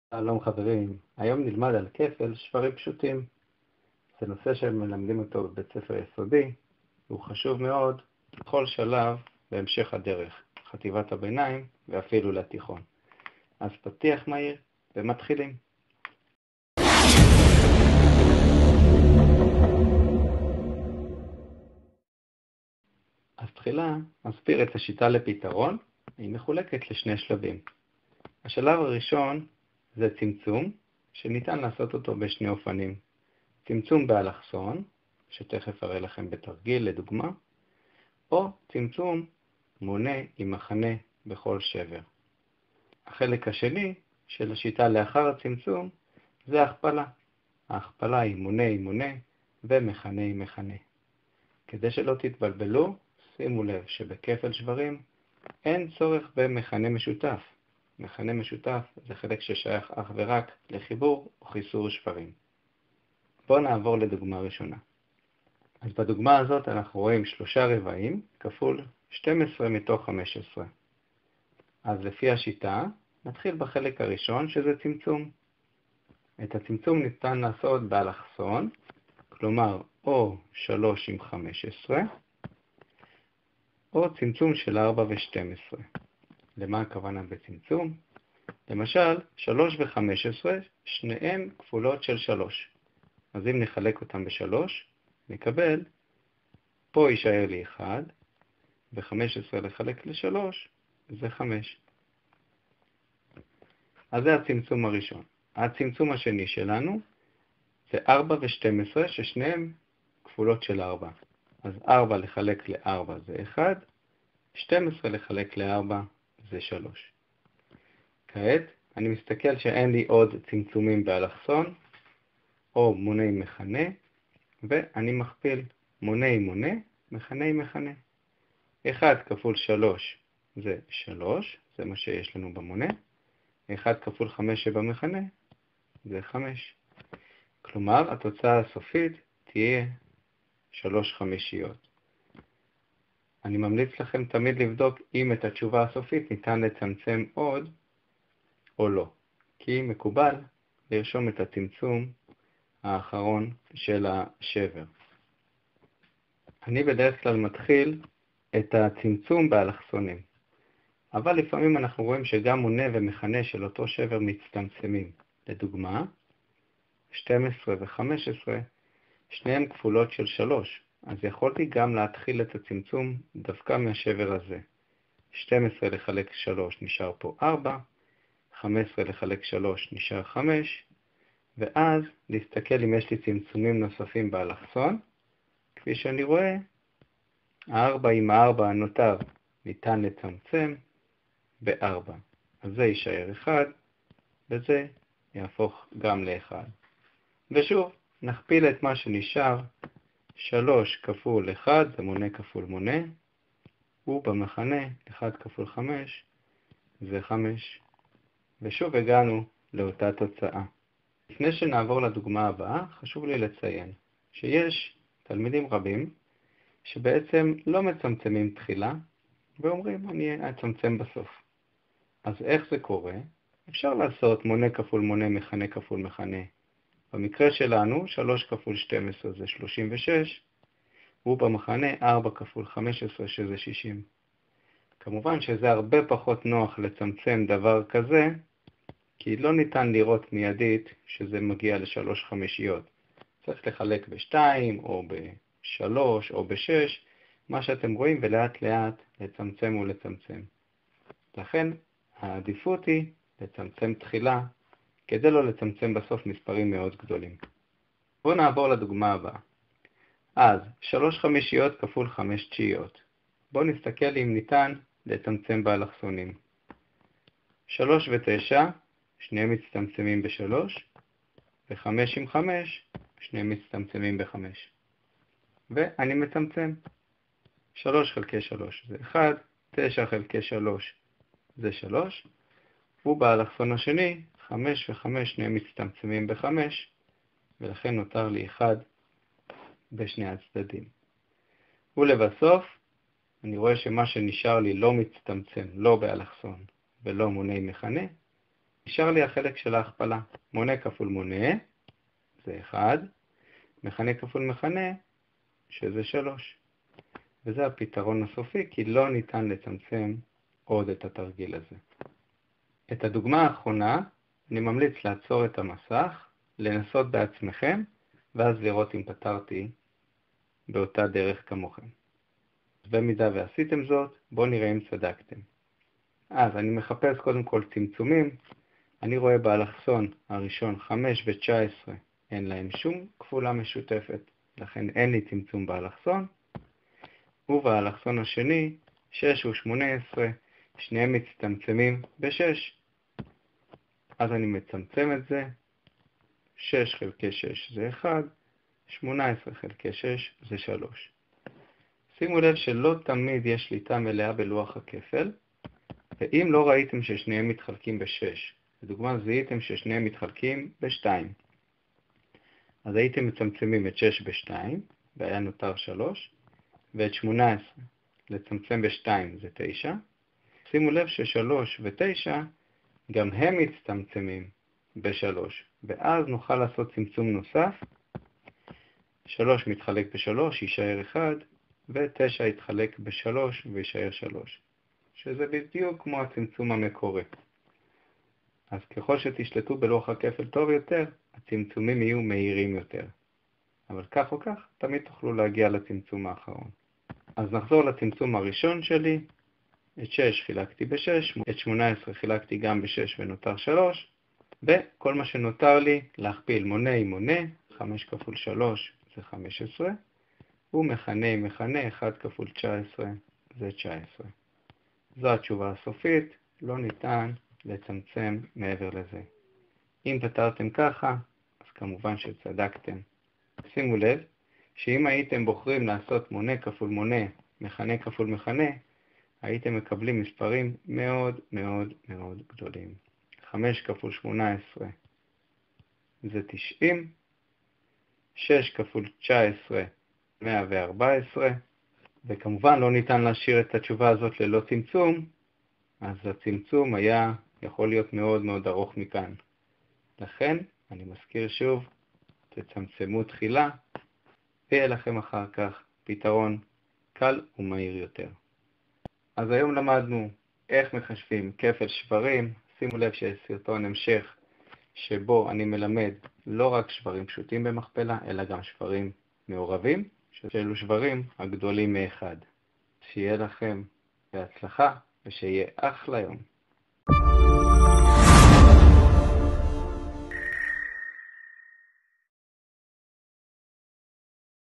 כפל שברים פשוטים // אין גבול | יסודי
כפל שברים הוא אחד הנושאים החשובים ביותר בבית הספר היסודי וחשוב לעשות אותו בצורה נכונה ויעילה.
והסרטון מלמד לפתור תרגילים ביעילות ובקלילות.

האימייל שלי לבקשות ושאלות נוספות